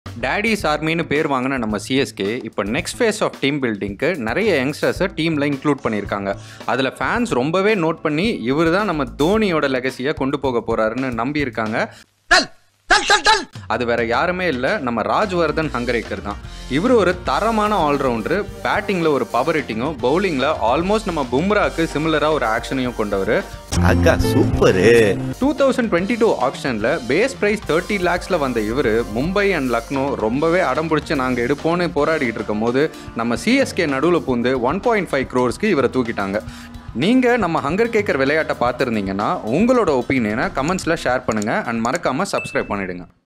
CSK அணிக்கு அடுத்த Dhoni ரெடியா ? - Rajvardhan Hangargekar
Rajvardhan Hangargekar - A all round youngster in CSK team is said the he is the best replacement for Dhoni. Do you agree? Share your comments and thought in the comment section.